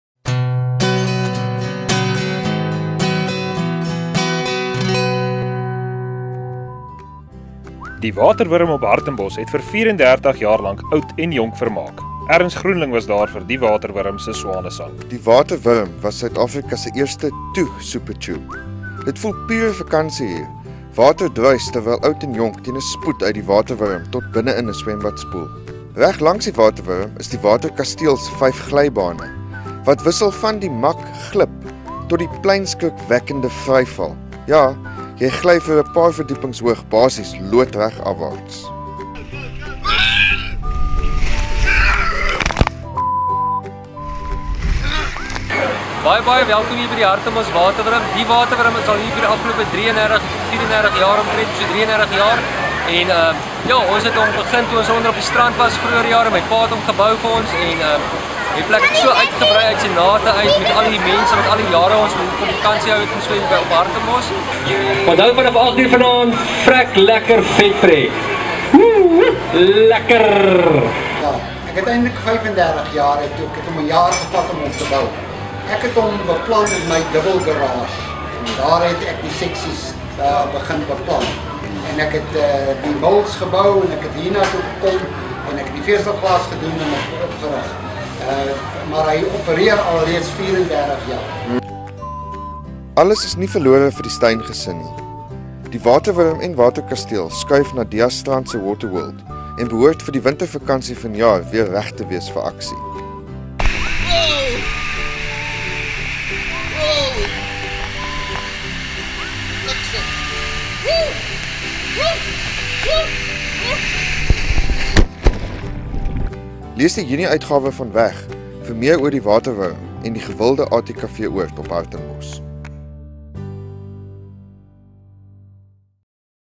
Viva die Waterwurm, viva!
Hartenbos se legendariese Waterwurm het in Januarie vanjaar oud en jonk vir oulaas vermaak. Erns Grundling het met die eienaar, Dirk Steyn, en sy pa gesels en self met ’n GoPro op Suid-Afrika se oudste “toe” super tube gaan gly. Erns het ook die skrikwekkende “Vryval”-glybaan by die Waterkasteel aangedurf en iets kwytgeraak wat die kinders se ore laat tuit het… Die Waterwurm het intussen geskuif na die Diasstrand. Lees die Junie-uitgawe van Weg vir meer oor die Waterwurm en die gewilde ATKV-oord op Hartenbos.